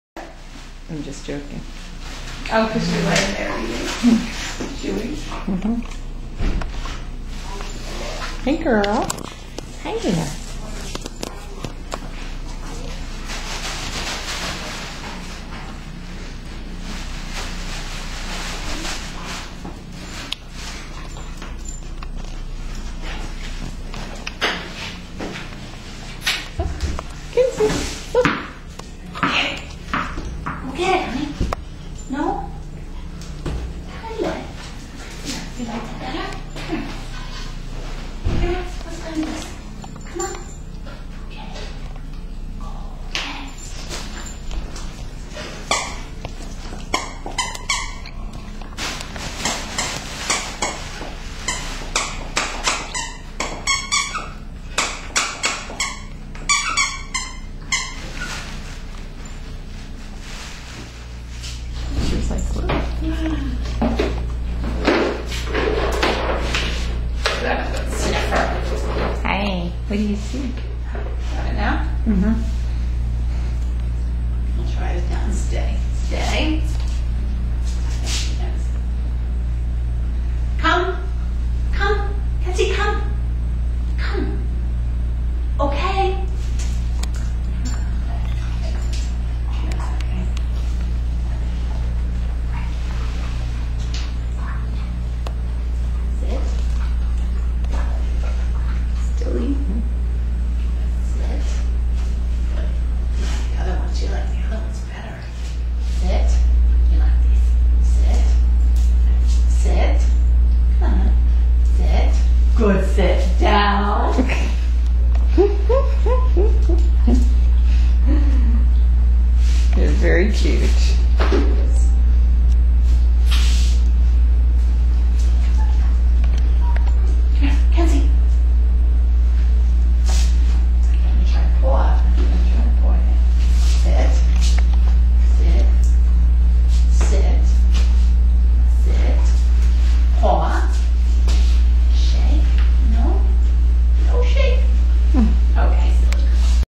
Kensey.wmv
Kensey is available for adoption at the Monmouth County SPCA.  She has a new home!